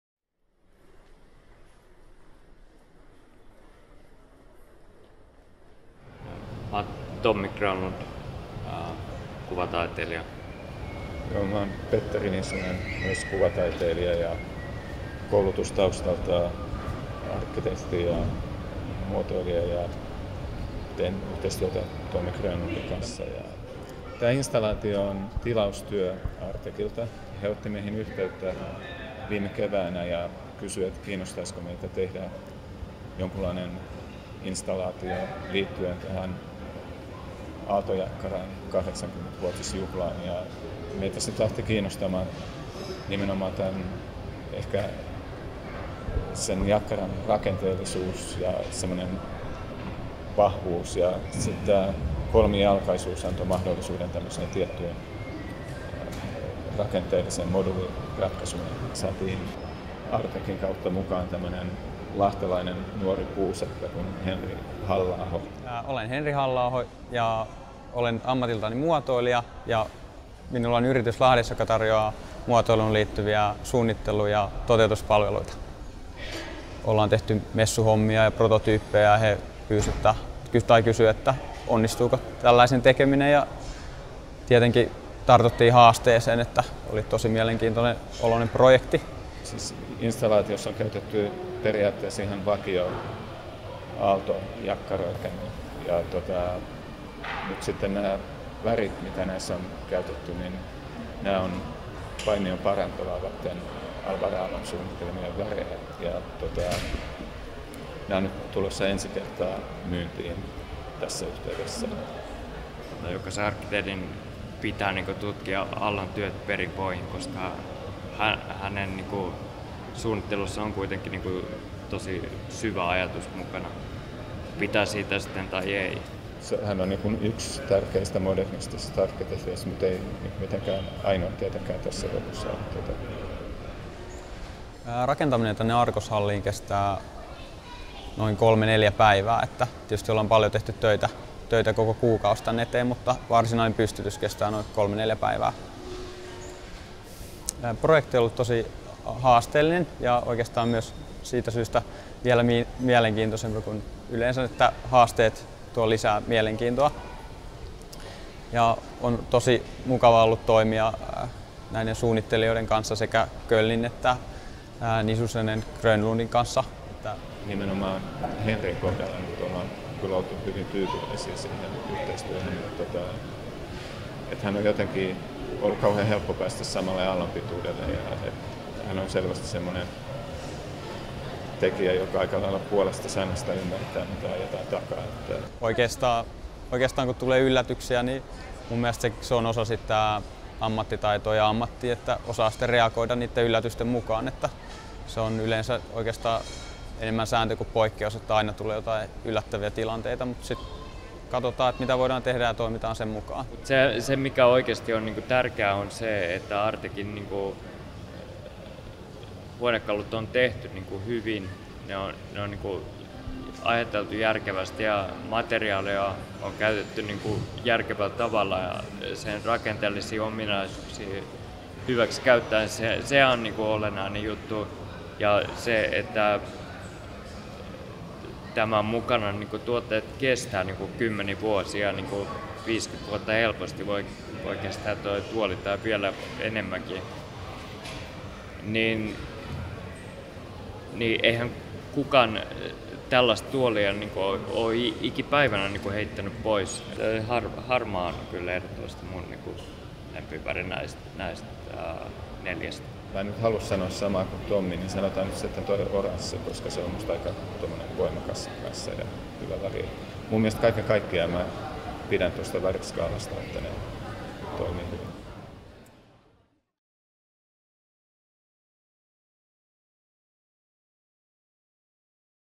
Artek Stool 60 -installaatio Stockmann Argos-hallissa
Syyskuussa Argoshallissa ihastellaan kansainvälisesti arvostetun taiteilijaduo Grönlund & Nisusen rakentamaa spektaakkelimaista tilateosta, joka koostuu lähes 400:sta erivärisestä Aalto-jakkarasta. // I september ställer den internationellt ansedda konstnärsduon Grönlund & Nisunen ut ett spektakulärt verk av närmare 400 Aaltopallar i olika färger i Argoshallen.